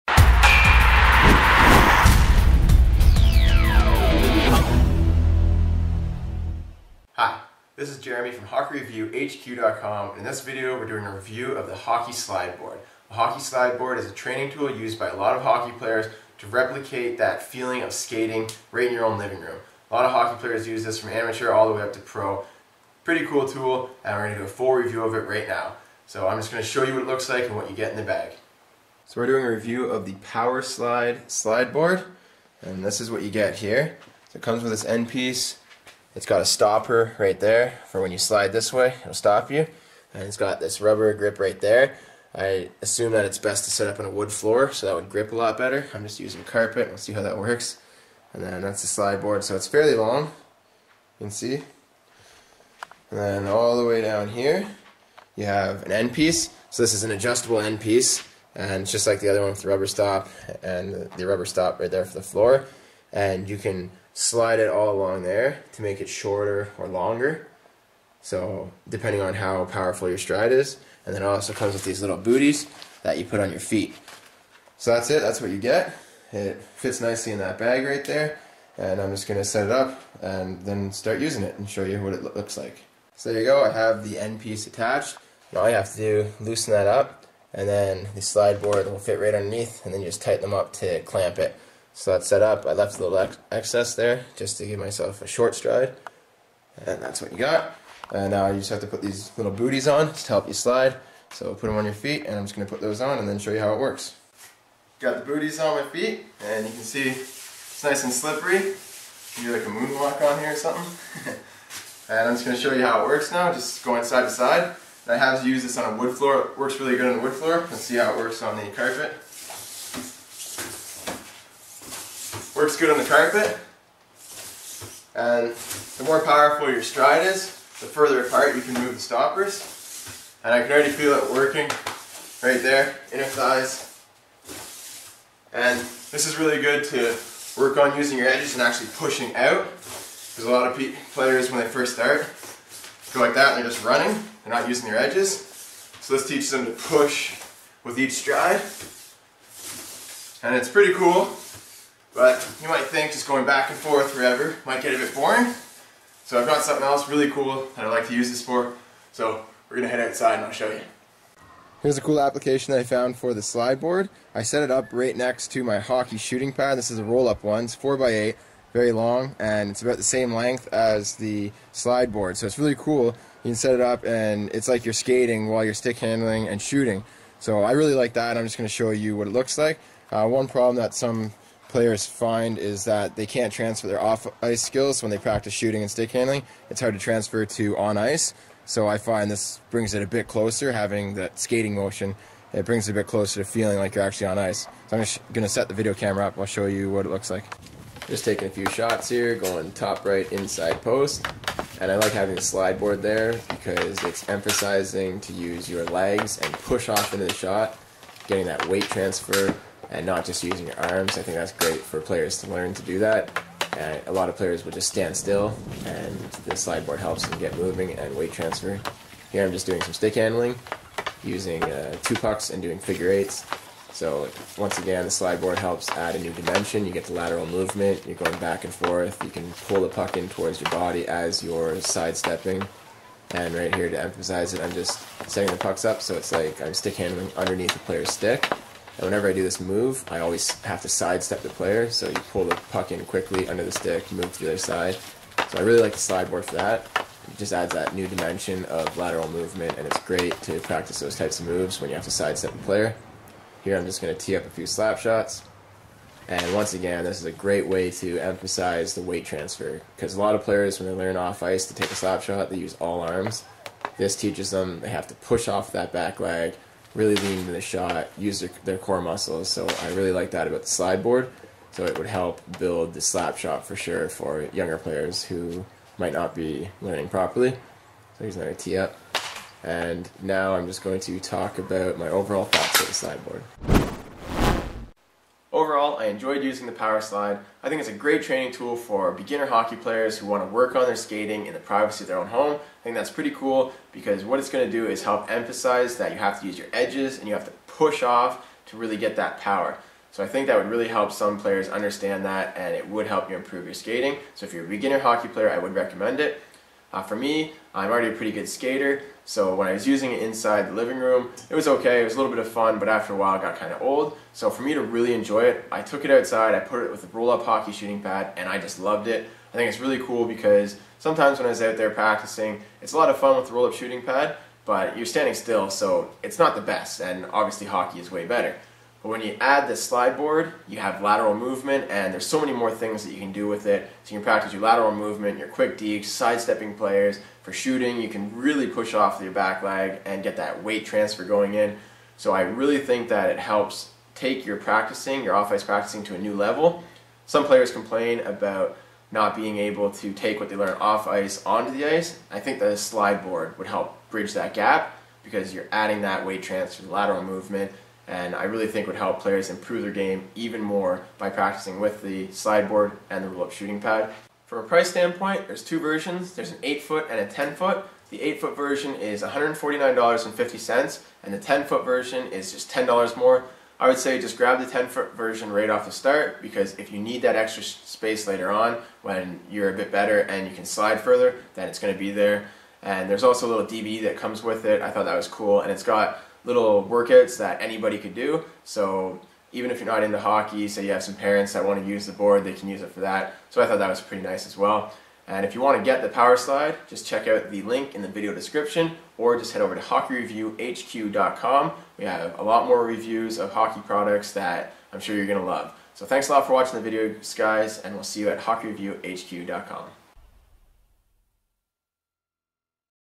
PowerSlide Board Review from HockeyShot.com
PowerSlide Board Review from HockeyShot.com. Have you ever wanted to work on your skating at home? With the Powerslide hockey slideboard you can practice your stride in your own living room! The powerslide is a neat hockey training tool that is supposed to replicate the feeling of skating. In this review we test it out, and show you how it works.